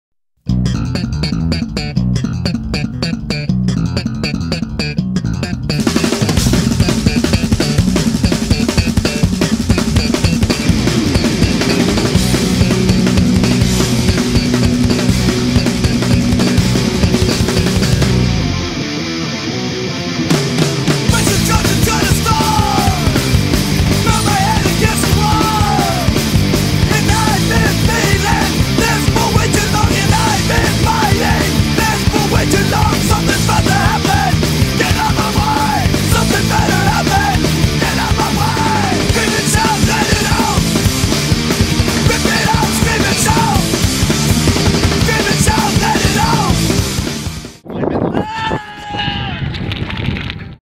Saut à L'Elastique 50 m - Tethwinn 2016 - Antipodes Millau // Saint Eulalie de Cernon
Sainte Eulalie de Cernon
Août 2016
Antipodes - 50 mètres

Vous voulez soutenir Real Life Prod ? Rendez-vous sur Tipeee !